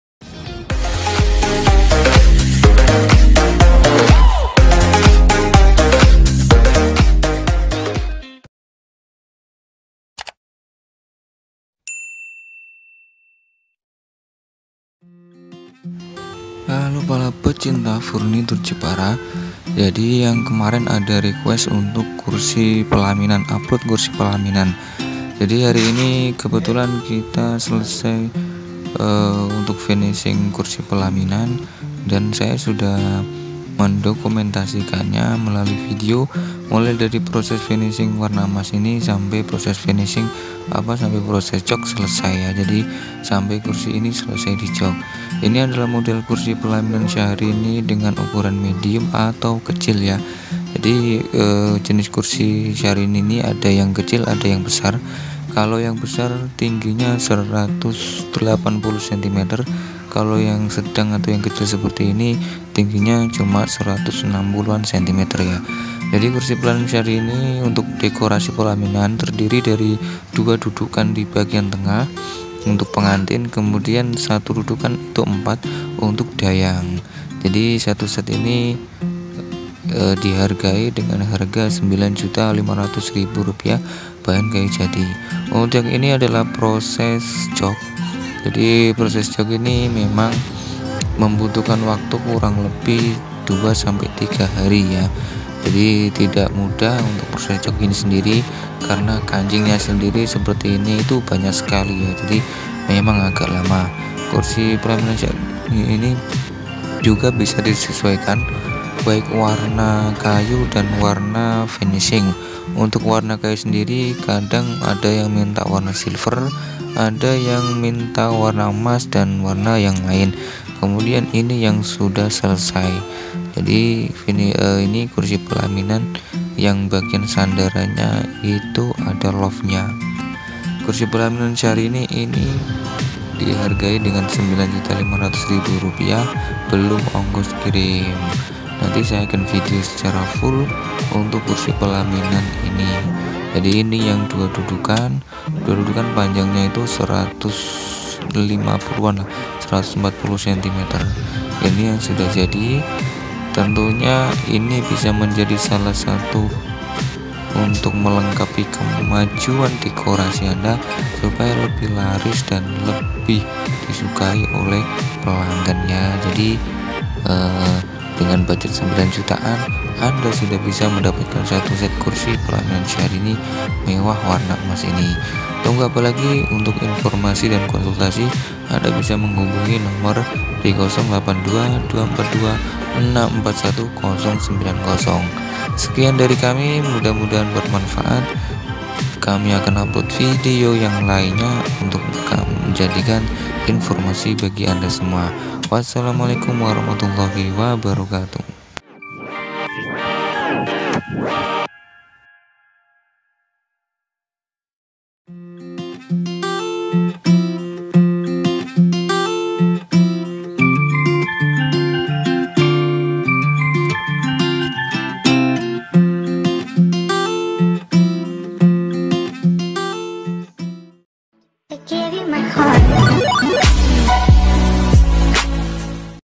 Kursi pelaminan Jepara SYAHRINI KECIL- Dekorasi pelaminan
Kursi pelaminan syahrini kecil, kursi pelaminan warna emas anda juga bisa menyesuaikan jika ingin warna lain.
Kursi pelaminan harga terjangkau.